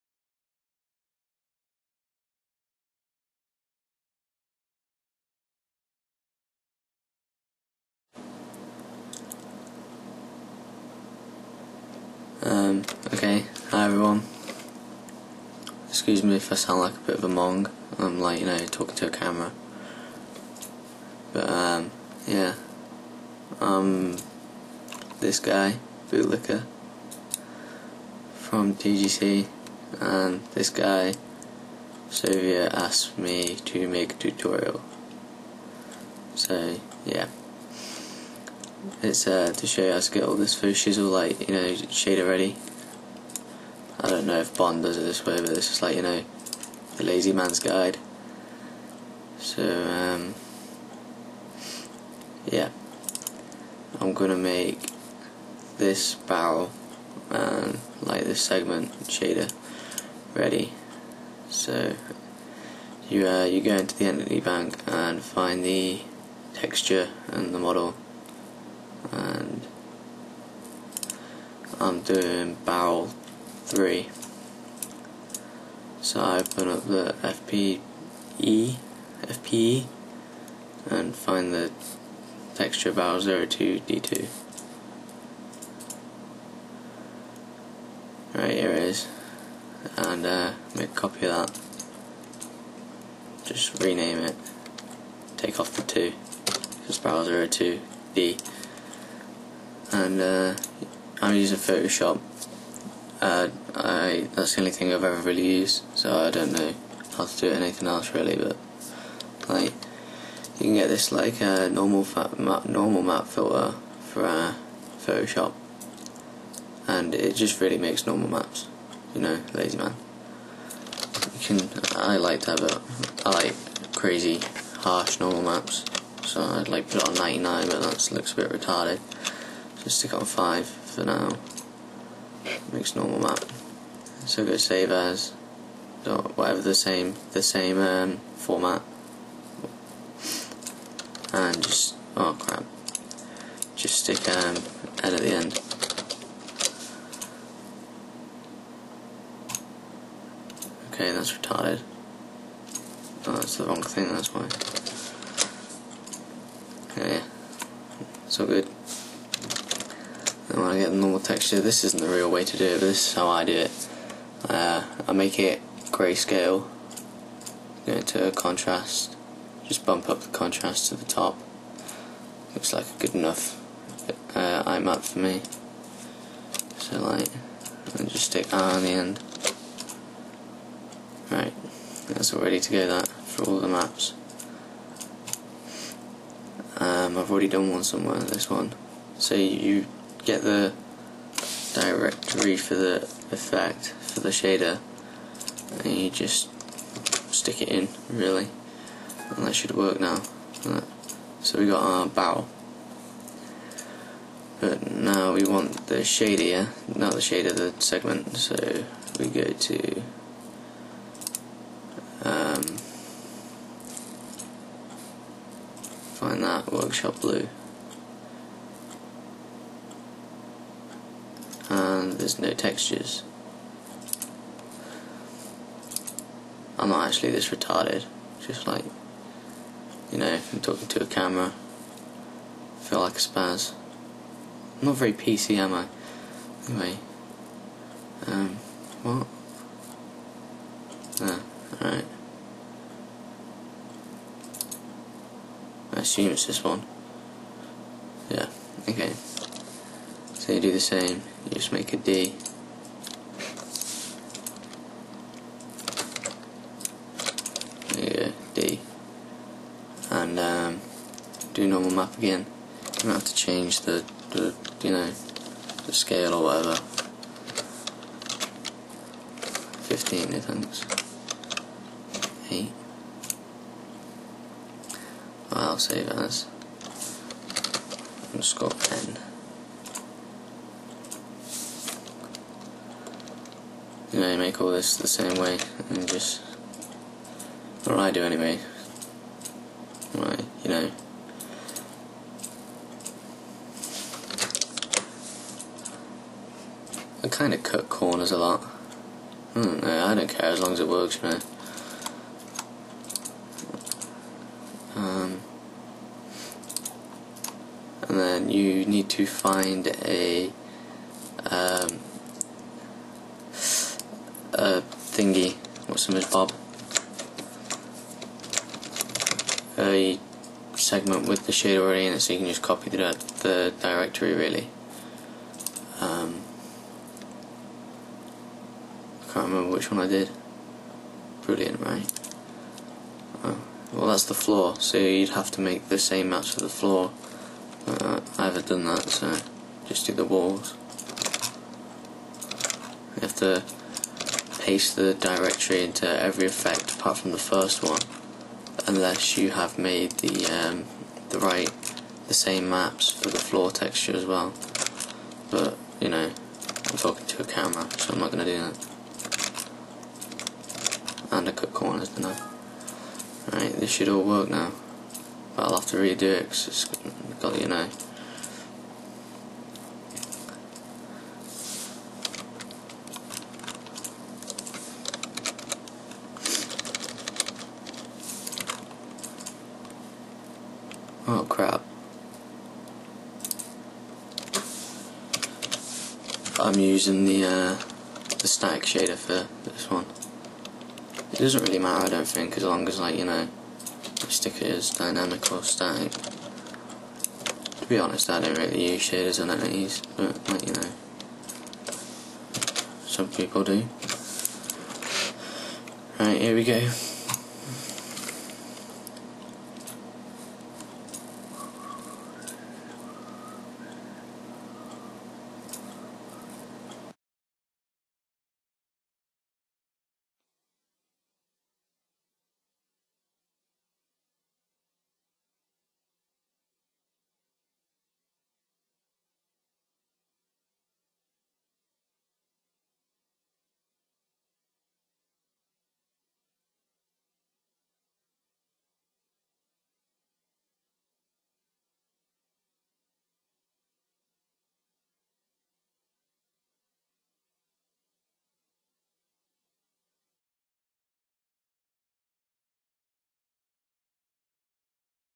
Making Segments and Entities Shader Ready
Here's  my tutorial on how to create the right maps for a segment or entity to work with shaders in FPS Creator. Shaders used are created by Bond1 from TGC.

You should have a:

_D
_D2
_I
_N

for the shader to work correctly